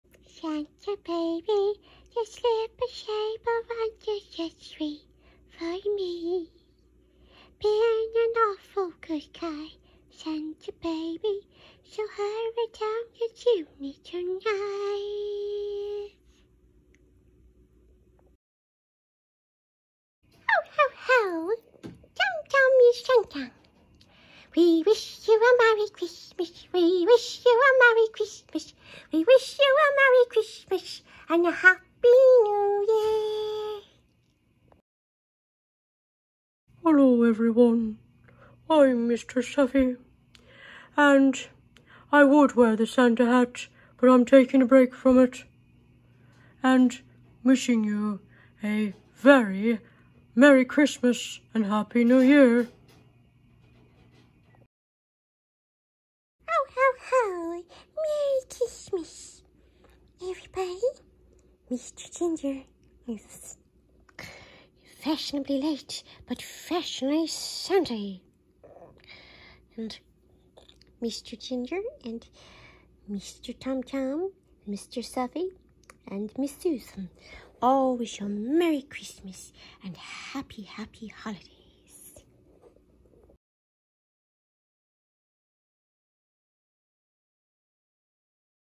Merry Christmas From Susans Fur N Fin Babies and AI Art
Merry Christmas And Happy Holidays Santa HoHoHo W/ Susans Fur N Fin Babies
(Mr. Ginger - baby bunny and therapeutic emotional support animal, Mr. Tom Tom - elderly hamster and drummer with .05A toothpicks, and Mr. Sapphire/Blueberry - blue betta and forever fin friend) and Custom Festive AI Art.